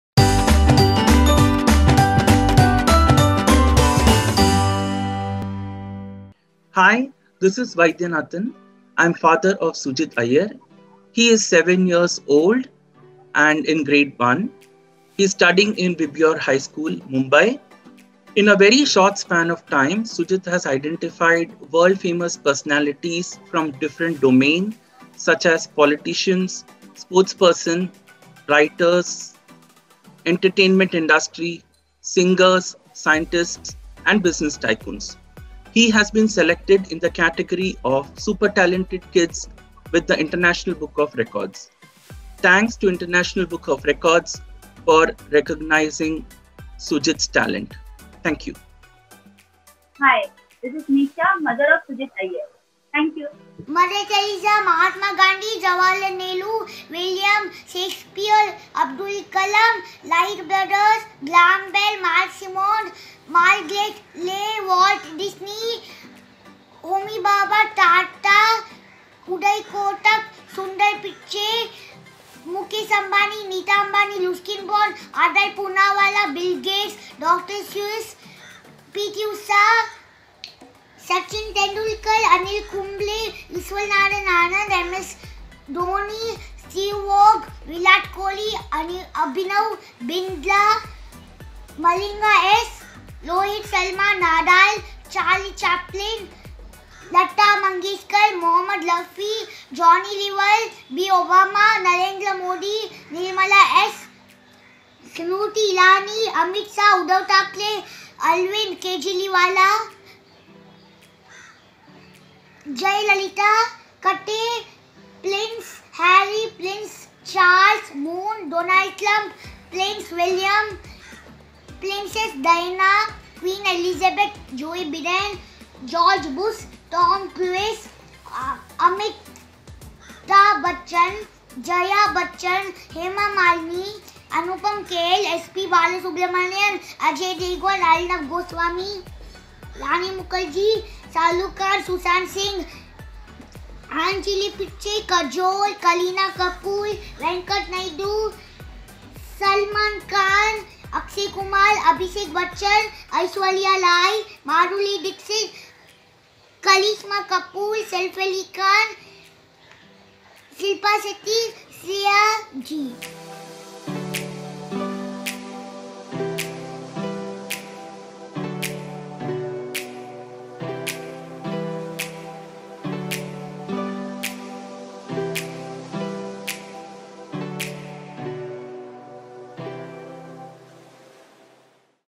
SUJIT IYER - A MEMORY POWER KID
SUJIT IYER is a Memory Power Kid from Mumbai (Maharashtra) India. 7 years Sujit has a sharp memory to memorize anything quickly. He identified 78 world famous personalities in 1 minute 49 seconds.
Thus, International Book of Records awarded him with "SUPER TALENTED KID" title on 1st February 2021 and found him "ONE IN A MILLION".

Welcome to the official Super Talented Kid YouTube channel! See the latest awards and stories of these Super Talented Kids.